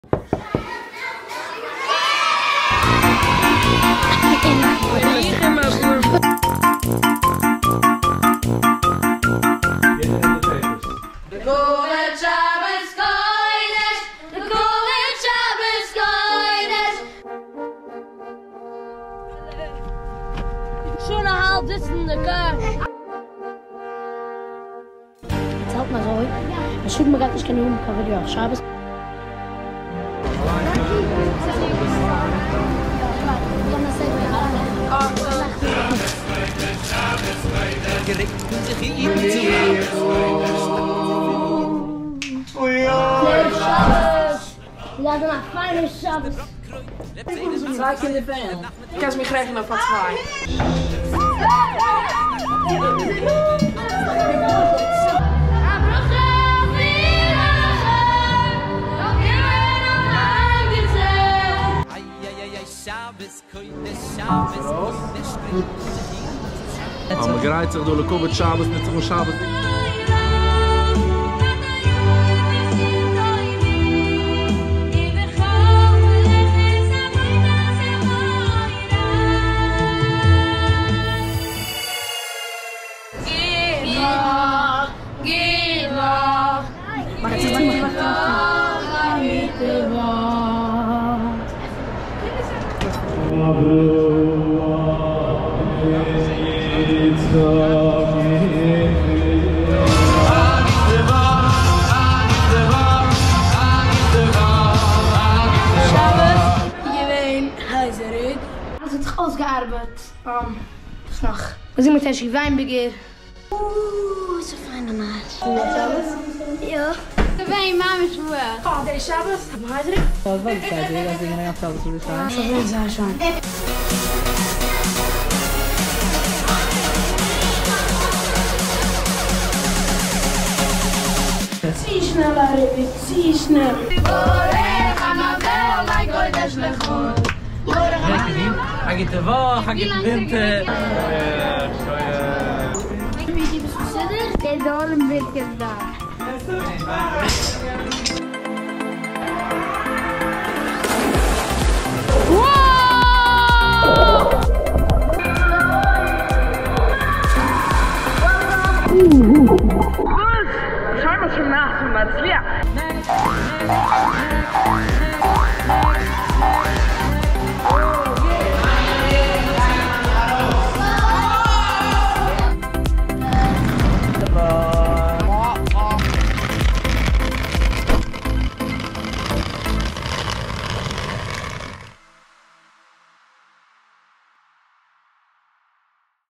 Yiddish Nachas’dig Ep. 02 - A Shabbes at the Armon Hotel in Stamford Ct with Families of Rayim.
We're proud to present to you the 2nd Episode in the brand new series called "Yiddish Nachas'dig" premium selected footage filmed behind the scenes at events where the Kinderlach of Yiddish Nachas performed, just to show you some small glimpse of the effort and work the kids put into each and every performance starting from when the gig gets confirmed till the drive home afterwards.

Episode 02 was filmed at a gateway Shabbes for the family’s of the well known Rayim Org where the Yiddish Nachas kids were brought to enhance the Shabbes meals & Davening.

Filmed & Edited by Volvi Spitzer